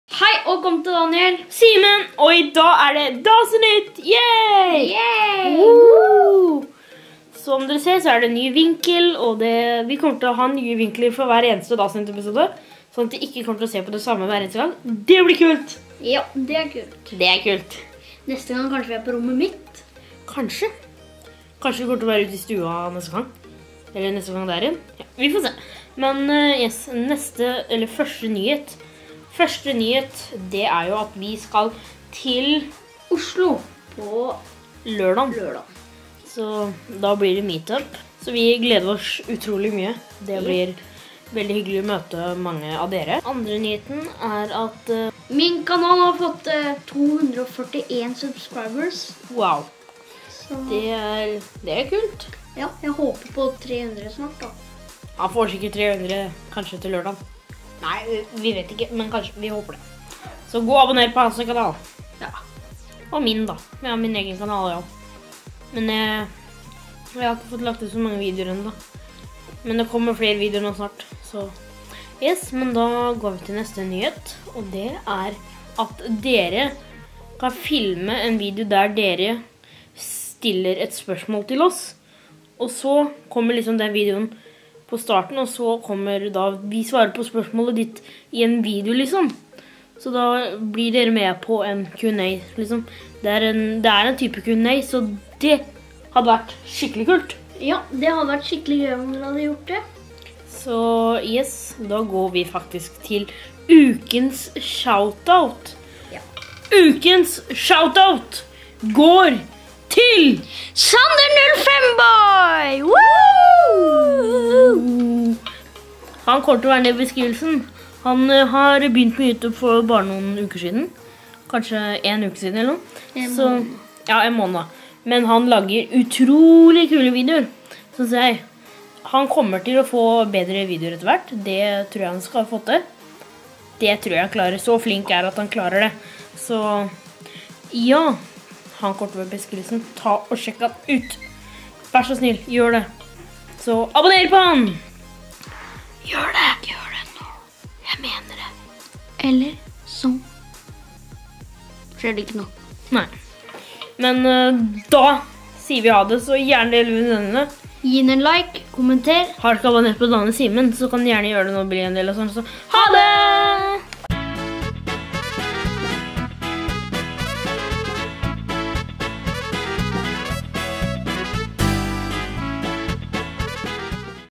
Blir det Oslo? | Dasinytt | episode 42
I dag er det Dasinytt, Vi avgjører om det Blir noe Oslo tur!

✔️Har du lyst på en T-skjorte!

           Adresse ▶️
     Kjellestadbakken 51
         3960 Stathelle

✔️Her streamer vi ▶️